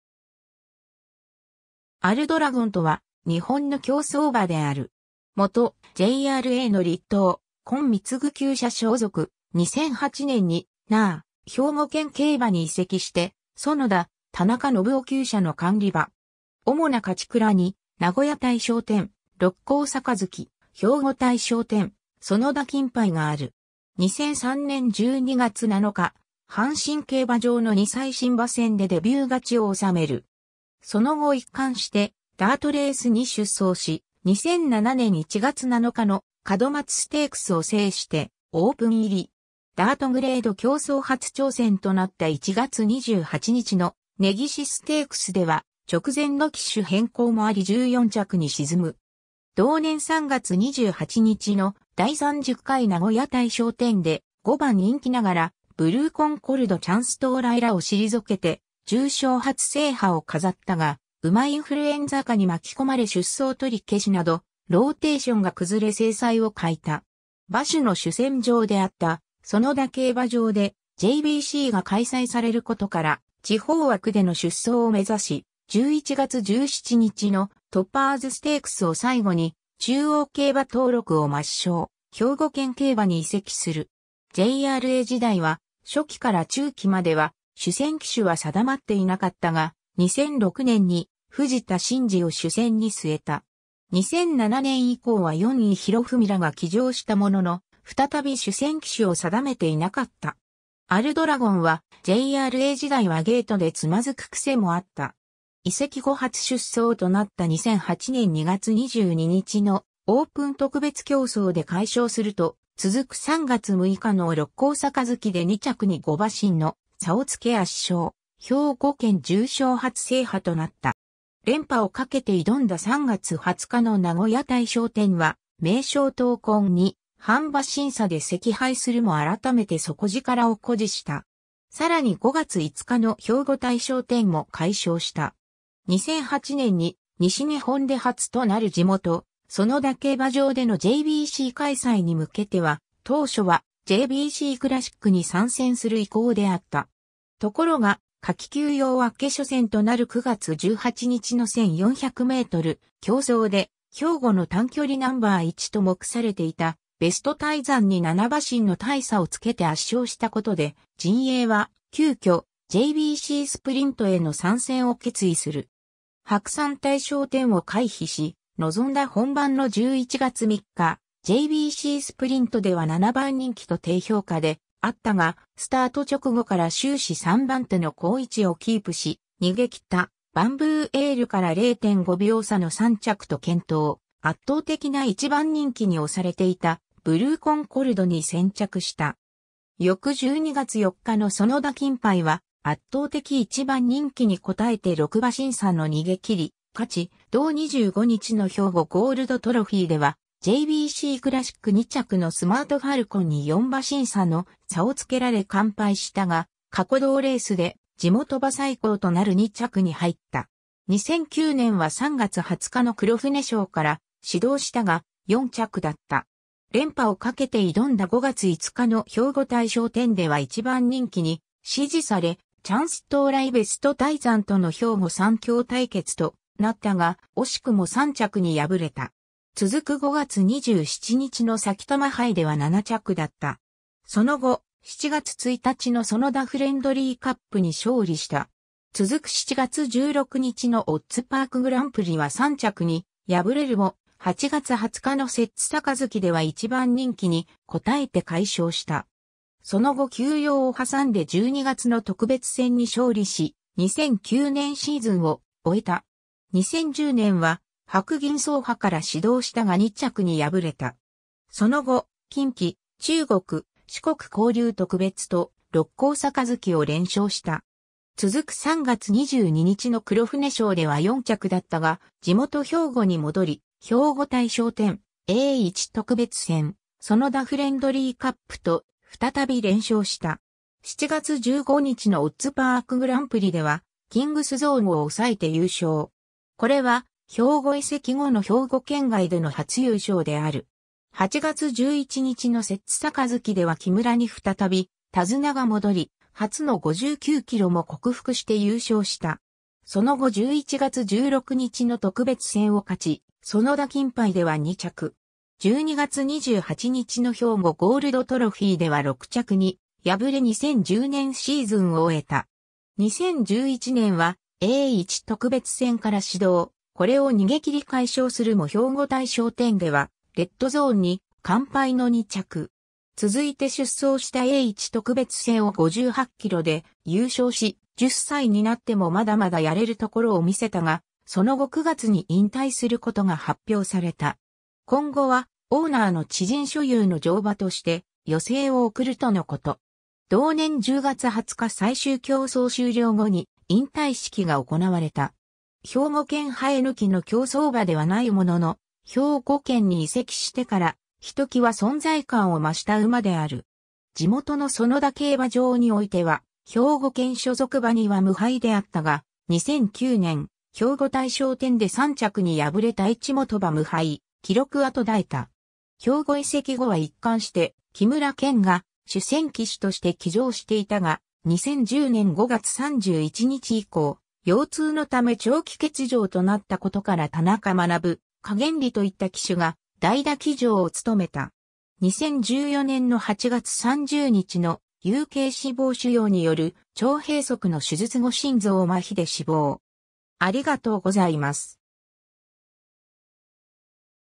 アルドラゴン
アルドラゴンとは日本の競走馬である。
元JRAの栗東・昆貢厩舎所属、2008年にNAR・兵庫県競馬に移籍して園田・田中範雄厩舎の管理馬。
おもな勝ち鞍に、名古屋大賞典(JpnIII・2007年)、六甲盃、兵庫大賞典、園田金盃(いずれも2008年)がある。
 2003年12月7日、阪神競馬場の2歳新馬戦でデビュー勝ちを収める。
その後一貫してダートレースに出走し、2007年1月7日の門松ステークスを制してオープン入り。
ダートグレード競走初挑戦となった1月28日の根岸ステークスでは直前の騎手変更もあり14着に沈む。
同年3月28日の第30回名古屋大賞典で、5番人気ながらブルーコンコルド・チャンストウライらを退けて重賞初制覇を飾ったが、馬インフルエンザ禍に巻き込まれ出走取り消しなど、ローテーションが崩れ精彩を欠いた。
馬主の主戦場であった園田競馬場でJBCが開催されることから　地方枠での出走を目指し、11月17日のトパーズステークス(10着)を最後に中央競馬登録を抹消、兵庫県競馬に移籍する。
JRA時代は初期から中期までは主戦騎手は定まっていなかったが、2006年に藤田伸二を主戦に据えた。
2007年以降は四位洋文らが騎乗したものの、再び主戦騎手を定めていなかった。
 アルドラゴンはJRA時代はゲートで躓く癖もあった。
 移籍後初出走となった2008年2月22日のオープン特別競走で快勝すると、続く3月6日の六甲盃で2着に5馬身の差をつけ圧勝、兵庫県重賞初制覇となった。
連覇を懸けて挑んだ3月20日の名古屋大賞典はメイショウトウコンに半馬身差で惜敗するも改めて底力を誇示した。
さらに5月5日の兵庫大賞典も快勝した。
 2008年に西日本で初となる地元・園田競馬場でのJBC開催に向けては、当初はJBCクラシックに参戦する意向であった。
ところが夏期休養明け初戦となる9月18日の1400メートル競走で、兵庫の短距離ナンバー1と目されていたベストタイザンに7馬身の大差をつけて圧勝したことで、陣営は急遽JBCスプリントへの参戦を決意する。
白山大賞典を回避し、臨んだ本番の11月3日、JBCスプリントでは7番人気と低評価であったが、スタート直後から終始3番手の好位置をキープし、逃げ切ったバンブーエールから0.5秒差の3着と健闘、圧倒的な1番人気に推されていたブルーコンコルド(4着)に先着した。
 翌12月4日の園田金盃は圧倒的1番人気に応えて6馬身差の逃げ切り勝ち、同25日の兵庫ゴールドトロフィー(JpnIII)では、JBCクラシック2着のスマートファルコンに4馬身差の差をつけられ完敗したが、過去同レースで地元馬最高となる2着に入った。
 2009年は3月20日の黒船賞(JpnIII)から始動したが、4着だった。
連覇を懸けて挑んだ5月5日の兵庫大賞典では1番人気に支持され、チャンストウライ・ベストタイザンとの兵庫三強対決となったが、惜しくも3着に敗れた。
続く5月27日のさきたま杯では7着だった。
その後、7月1日の園田フレンドリーカップに勝利した。
続く7月16日のオッズパークグランプリは3着に敗れるも、8月20日の摂津盃では1番人気に応えて快勝した。
その後休養を挟んで12月の特別戦に勝利し、2009年シーズンを終えた。
 2010年は白銀争覇から始動したが2着に敗れた。
その後、近畿・中国・四国交流特別と六甲盃を連勝した。
続く3月22日の黒船賞（JpnIII）では4着だったが、地元兵庫に戻り兵庫大賞典、A1特別戦、園田フレンドリーカップと再び連勝した。
7月15日のオッズパークグランプリではキングスゾーンを抑えて優勝。
これは兵庫移籍後の兵庫県外での初優勝である。
8月11日の摂津盃では木村に再び手綱が戻り、初の59キロも克服して優勝した。
その後11月16日の特別戦を勝ち、園田金盃では2着。
12月28日の兵庫ゴールドトロフィー(JpnIII)では6着に敗れ2010年シーズンを終えた。
 2011年はA1特別戦から始動、これを逃げ切り快勝するも兵庫大賞典ではレッドゾーンに完敗の2着。
続いて出走したA1特別戦を58キロで優勝し、10歳になってもまだまだやれるところをみせたがその後9月に引退することが発表された。
今後はオーナーの知人所有の乗馬として余生を送るとのこと。
同年10月20日最終競走終了後に引退式が行われた。
 兵庫県生え抜きの競走馬ではないものの、兵庫県に移籍してからひときわ存...